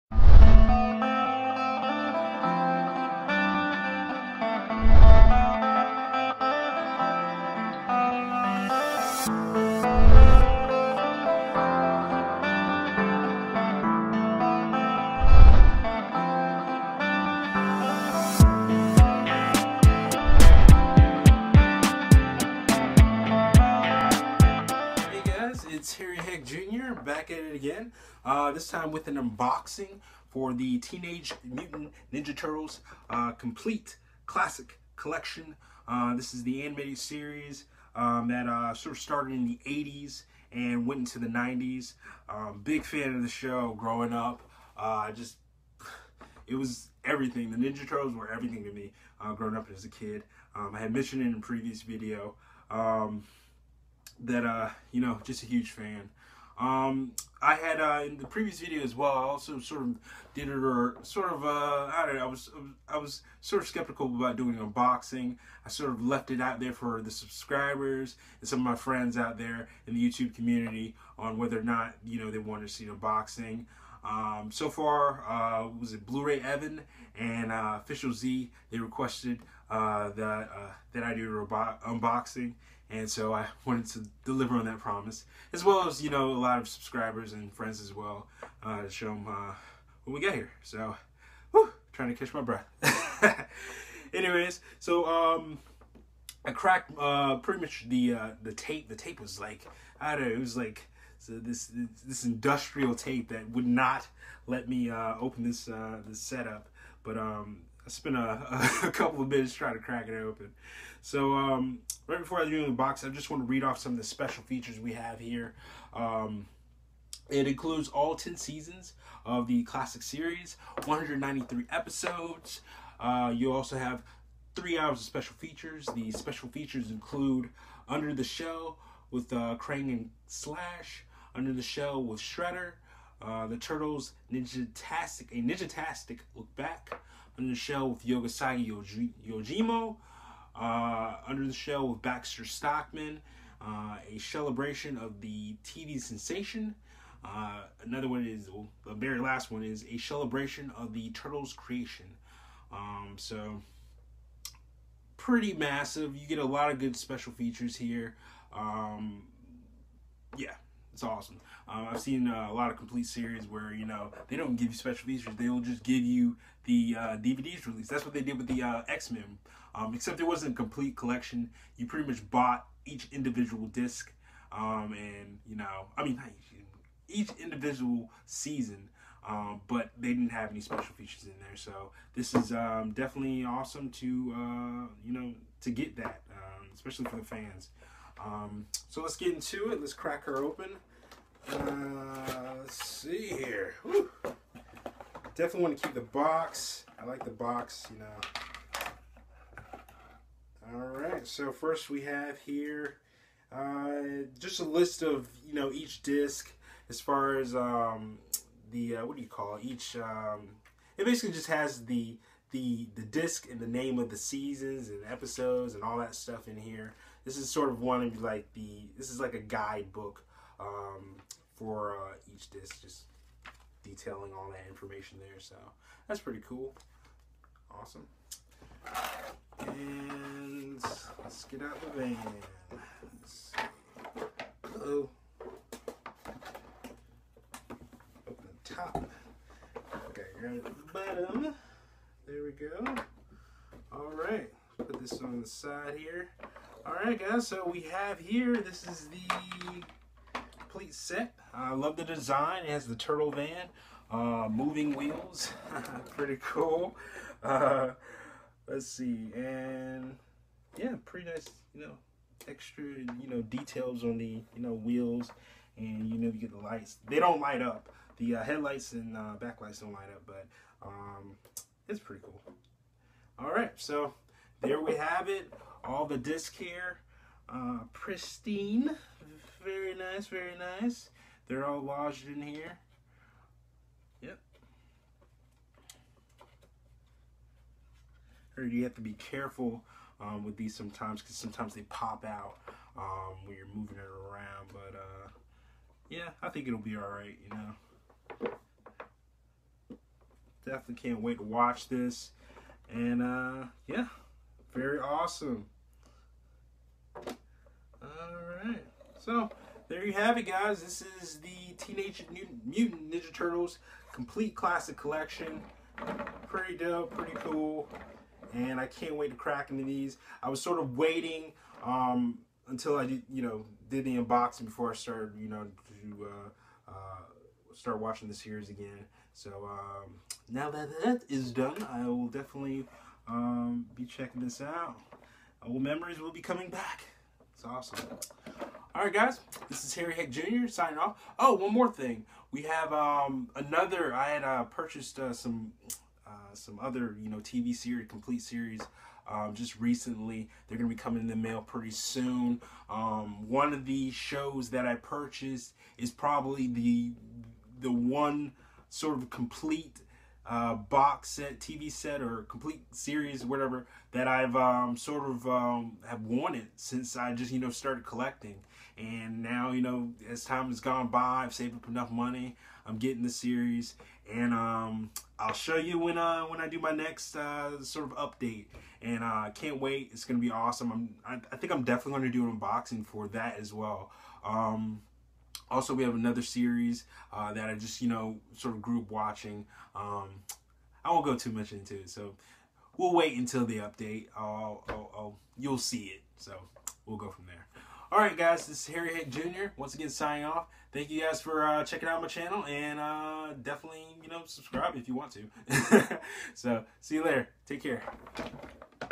Teenage Mutant Ninja Turtles Complete Series-Unboxing
In this video, I will be unboxing The Teenage Mutant Ninja Turtles Complete Classic Collection.